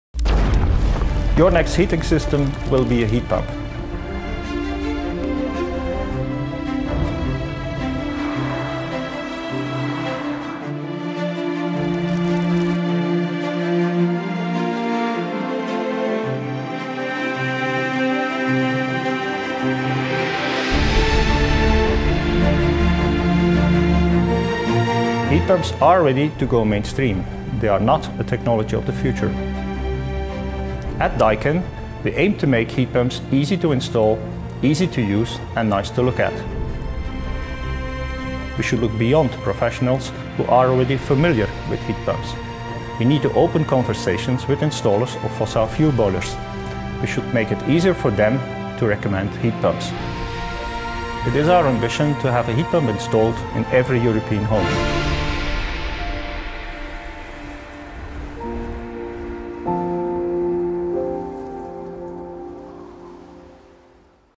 Your next heating system will be a heat pump
Patrick Crombez introduces the new Daikin Altherma 3 H HT, Daikin’s latest innovation to help accelerate home decarbonisation.